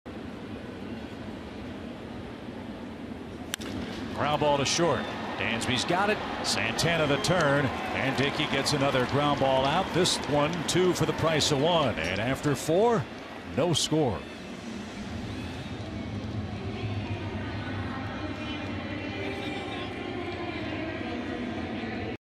ATL@MIA: Swanson starts a double play to end the 4th
Dansby Swanson fields a sharp grounder then flips it to Danny Santana as he fires it to Freddie Freeman for a double play to end the 4th